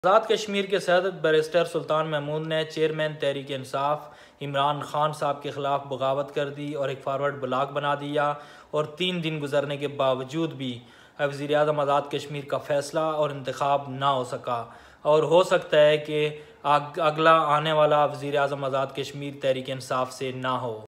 President Barrister Sultan Mehmood | Breaking News
President AJK Barrister Sultan Mehmood decision against Imran Khan.